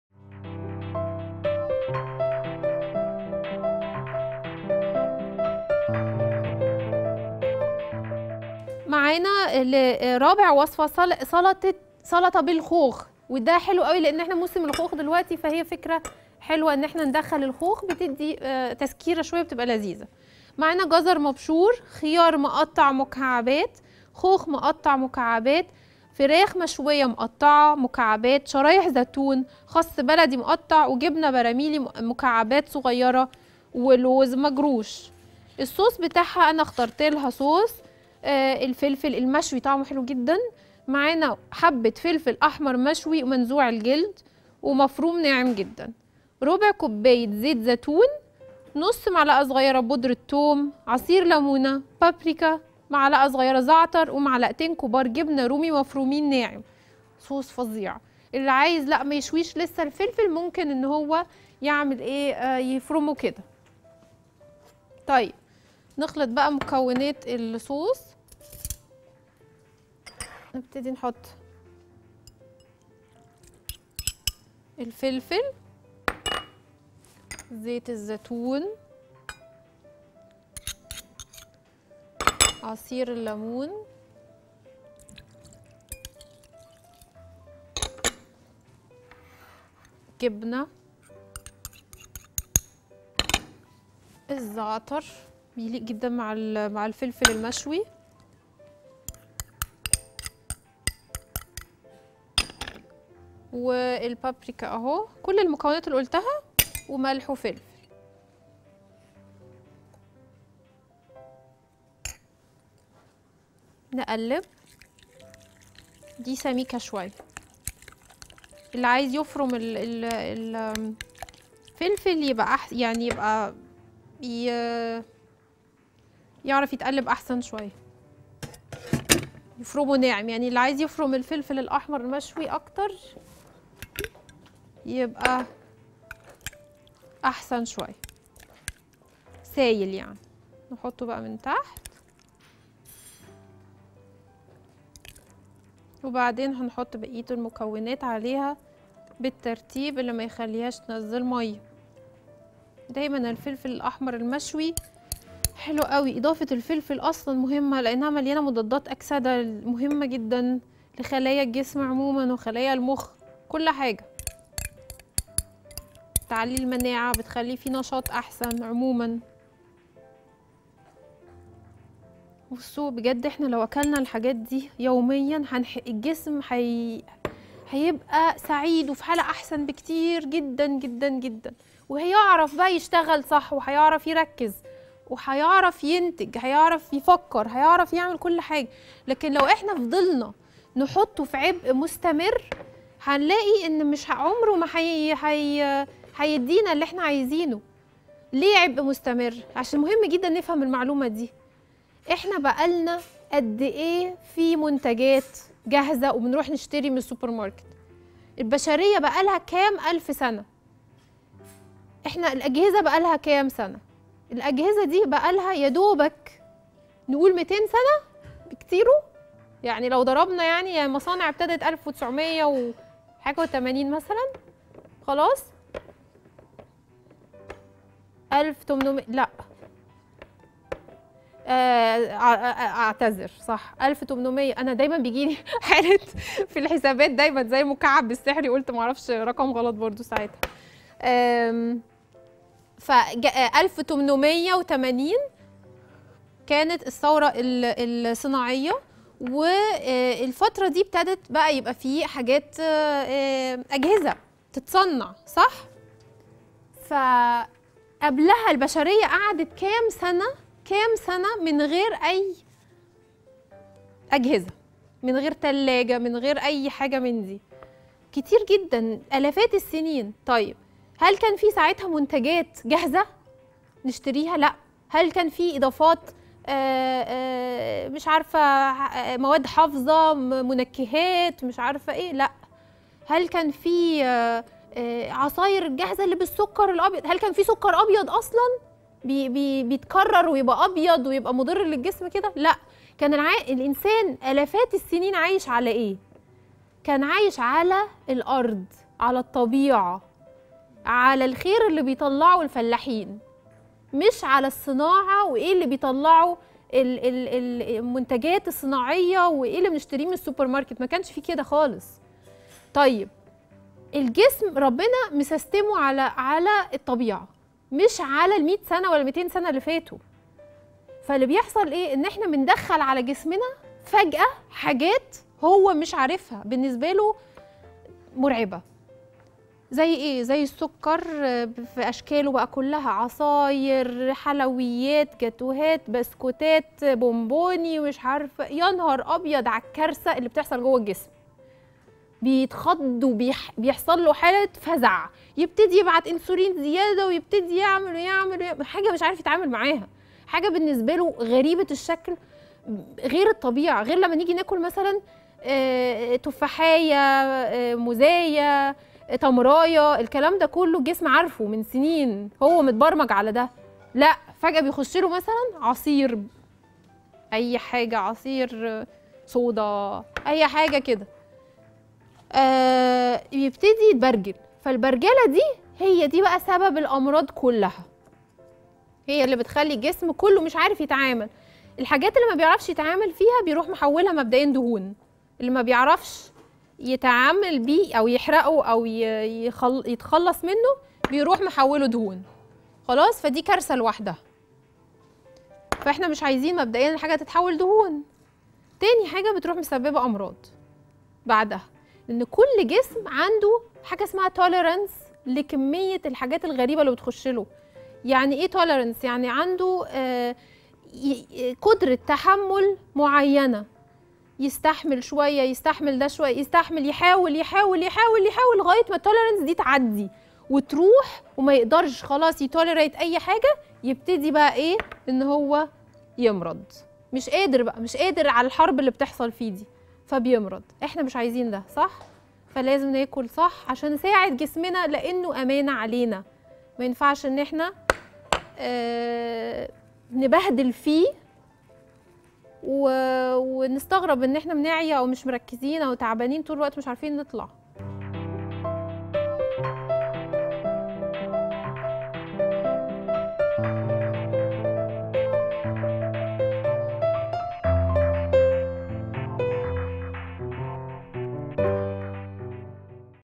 سلطة بالخوخ | سالي فؤاد
طريقة عمل سلطة بالخوخ   مع سالي فؤاد في حلو وحادق